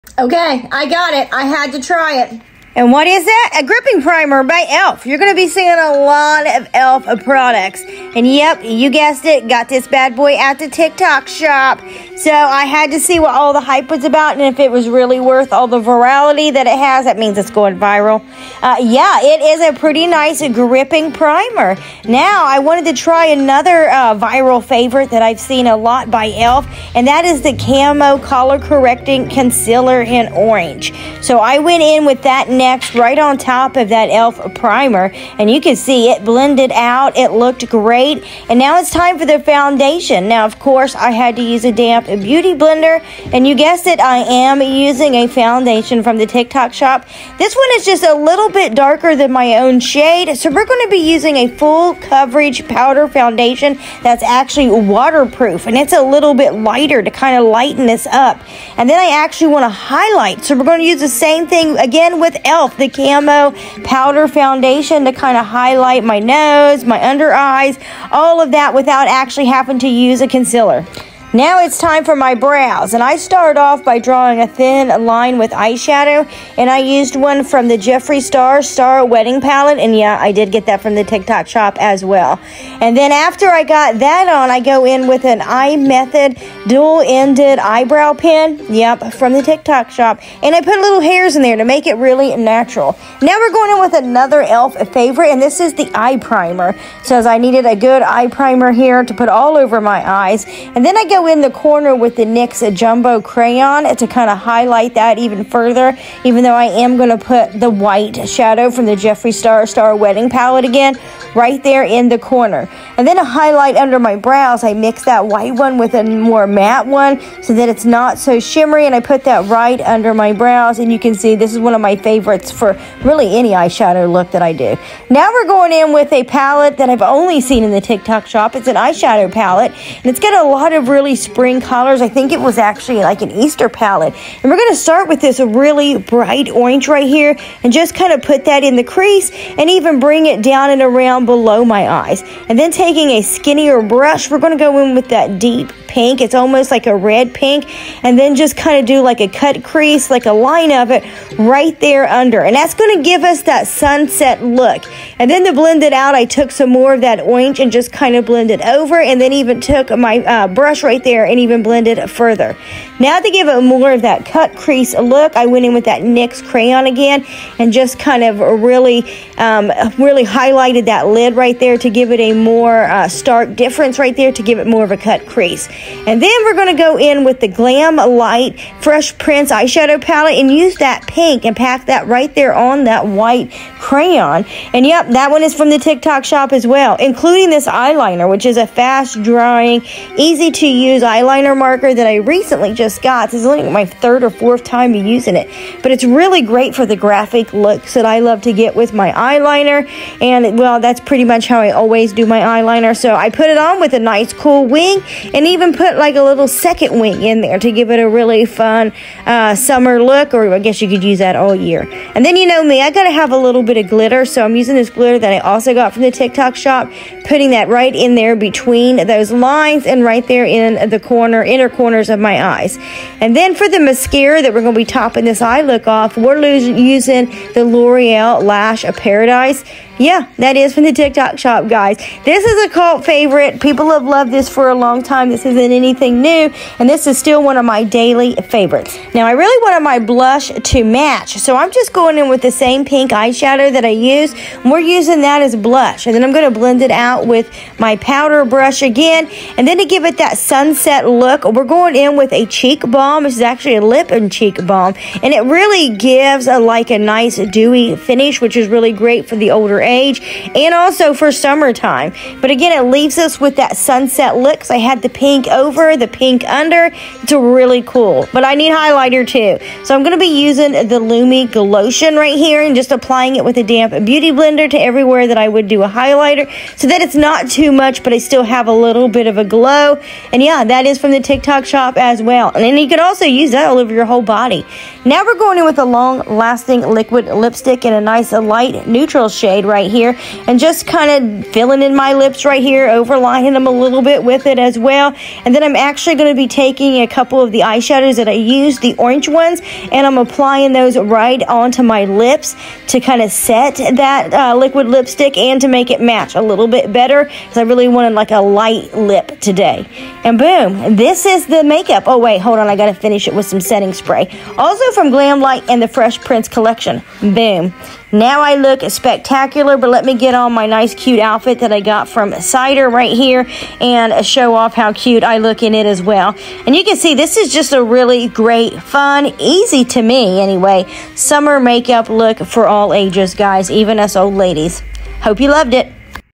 Summer Sunset Cut Crease Using Makeup from the TikTok Shop including Too Faced, E.L.F. and L’Oréal
Yup everything used in this loom came from the TikTok shop, and many straight froM the brands, L’Oréal, E.L.F. Jeffree Star, Too Faced and Glamlite are all on TikTok and available in the TikTok shop

To find links and the items featured, check out my TikTok, they are in my showcase, or find the videos that correspond with the item!

Have u bought any makeup from TikTok yet? If so what, and are u happy with ur purchase?